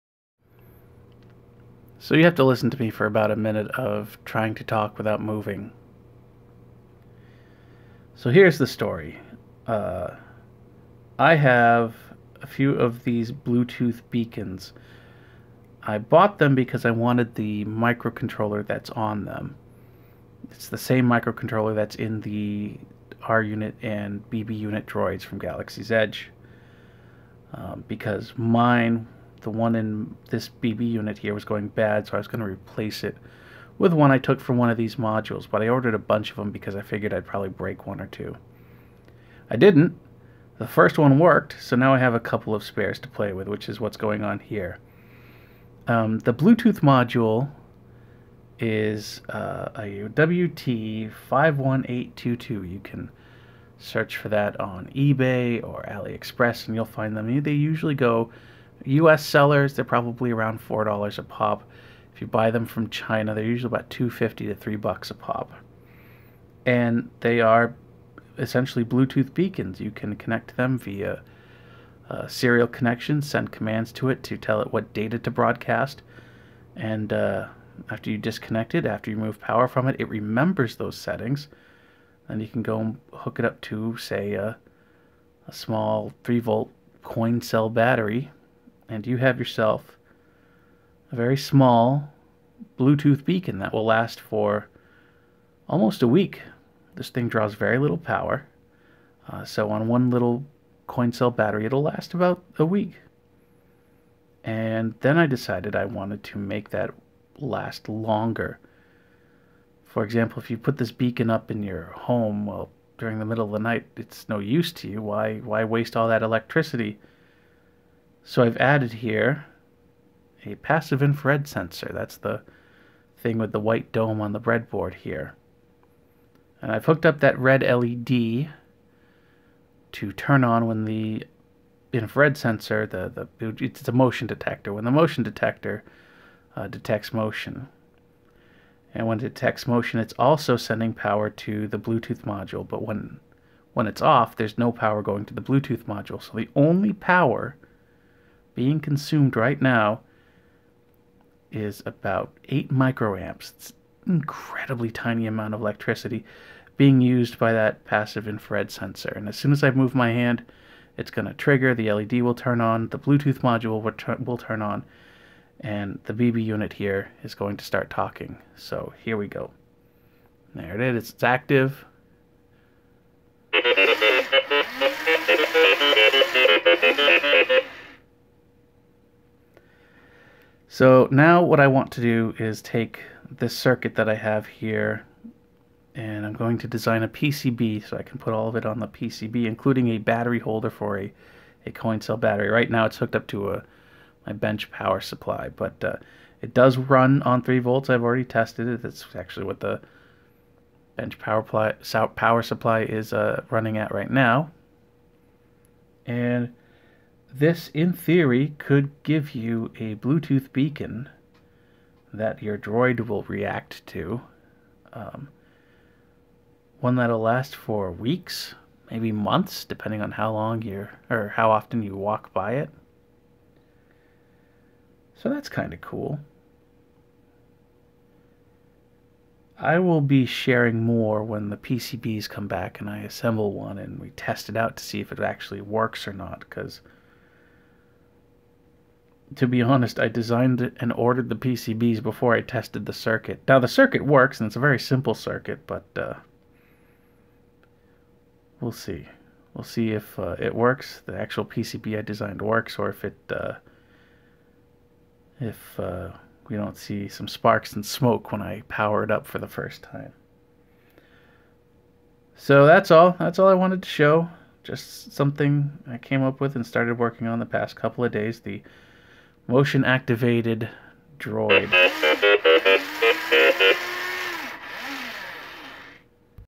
A Motion Activated Droid
A proof of concept using a motion activated bluetooth beacon to trigger a response from droids built at the Droid Depot in Galaxy's Edge. I have designed and ordered some PCBs to put all of this together onto a single board. Assuming it works, I'll post a video showing the PCB and a link to OSH Park where you'll be able to order it.

I have the bluetooth module soldered to breakout board I found and ordered through OSH Park so that I could use it with a breadboard. You can order that board here:

The PIR sensor I'm using is an AM312 which can be found on eBay.